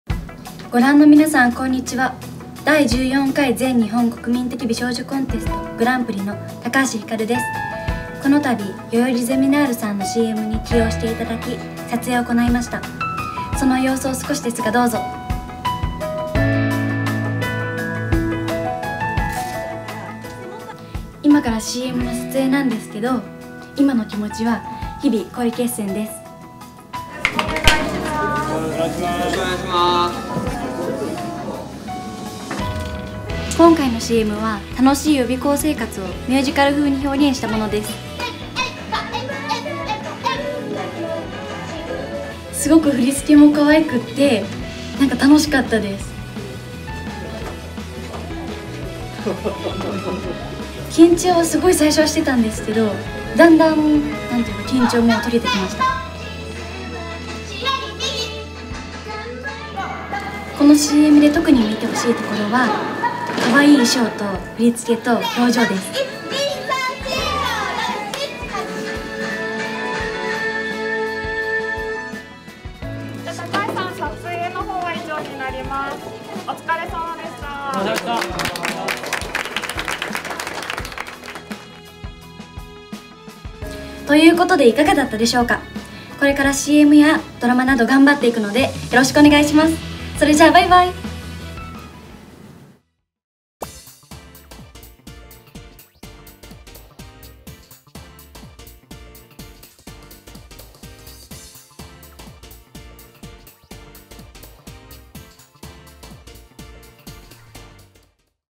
【髙橋ひかる】「代々木ゼミナール」ＣＭ撮影風景
第14回全日本国民的美少女コンテストグランプリの
髙橋ひかるが「代々木ゼミナール」さんのCMに出演中！
そのCMのメイキングの様子をご覧ください！！

「代々木ゼミナール」さんと髙橋ひかるをこれからも